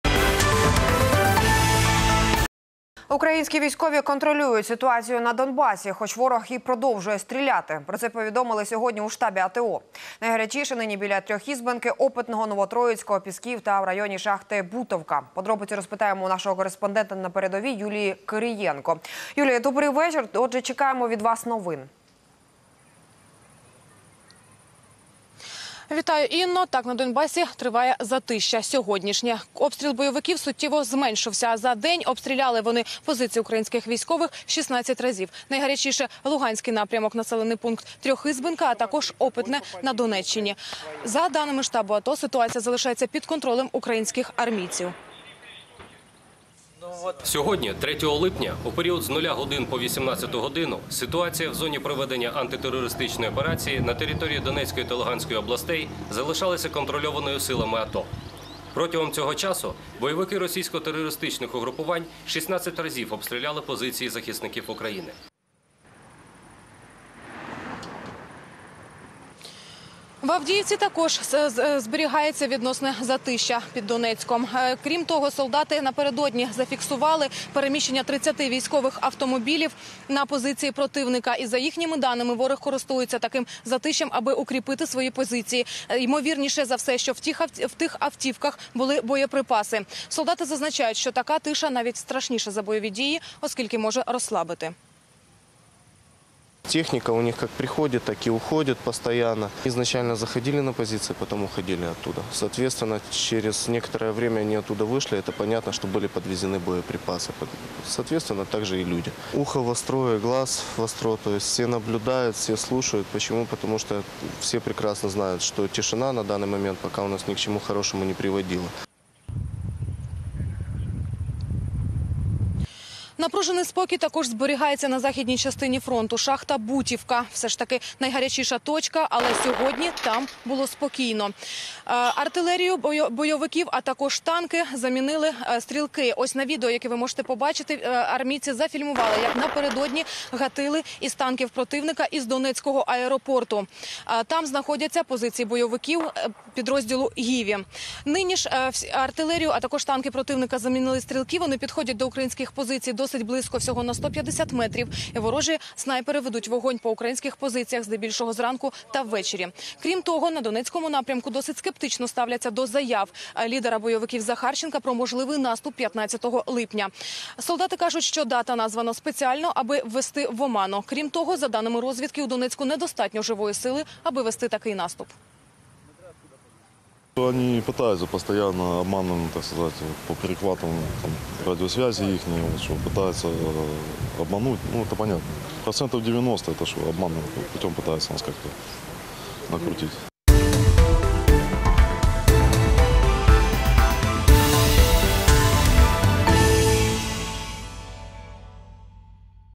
Боевики притихли: 16 обстрелов в сутки
Украинские военные контролируют ситуацию на Донбассе - хоть враг и продолжает стрелять - об этом сообщили сегодня в штабе АТО. Горячее всего сейчас у Трехизбенки, Опытного, Новотроицкого

Українські військові контролюють ситуацію на Донбасі - хоч ворог і продовжує стріляти - про це повідомили сьогодні у штабі АТО. Найгарячіше нині біля Трьохізбенки, Опитного, Новотроїцького